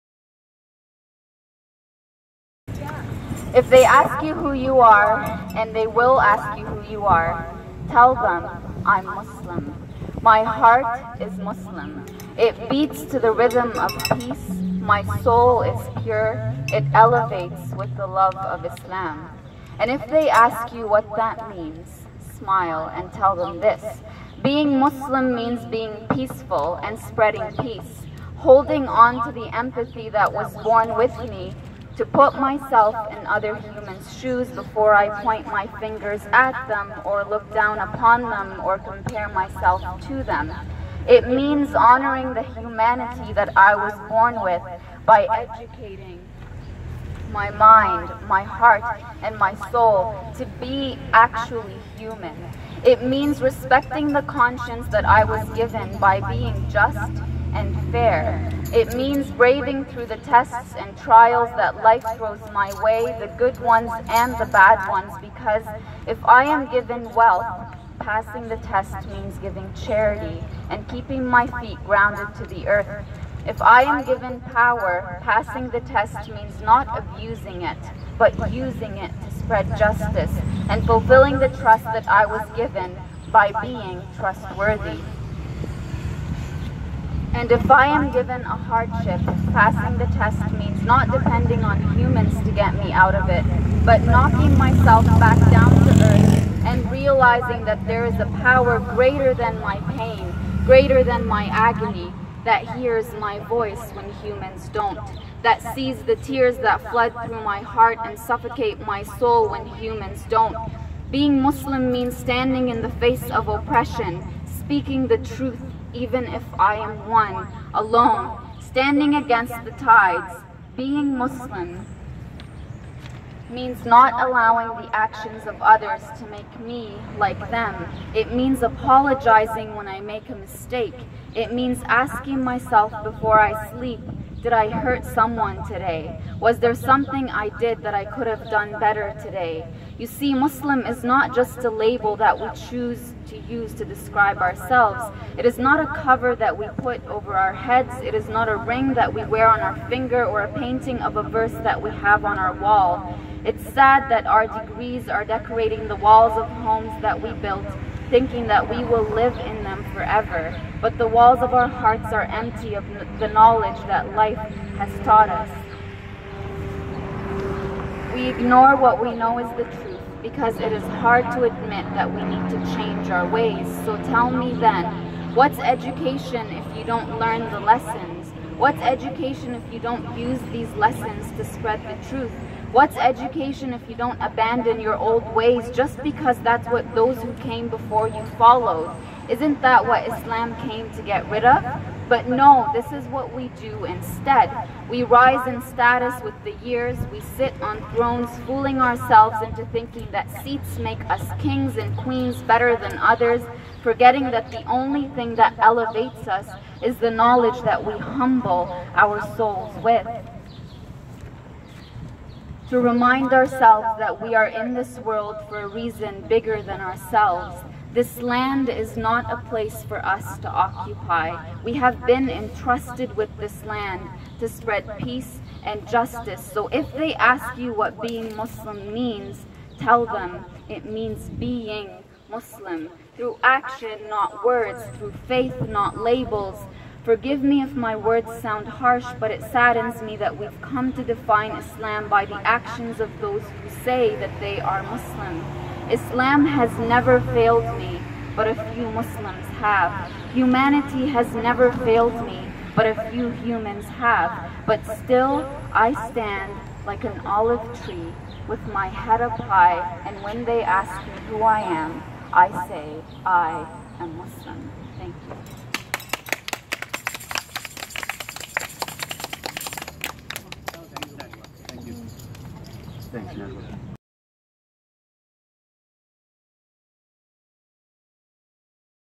What being Muslim means to me *Najwa Zebian*
A group of people gathered up in front of London City Hall to spew hatred against Muslims, so Najwa Zebian went there to tell them what Islam means to her.

"Don't doubt that wherever and whenever hatred towards any group is spewed, that I'll be there to fight for them.
Being visibly Muslim, people automatically assume sometimes that I speak only to and about Muslims. That is very wrong.
Silenced souls are everywhere. They don't come in one color, religion, faith, beliefs etc. I speak for humans. All humans." - Najwa Zebian

Filmed and Edited by Mike Roy
Recorded in London, Ontario Canada  (July 22nd 2017)
The Indignants/Revolution News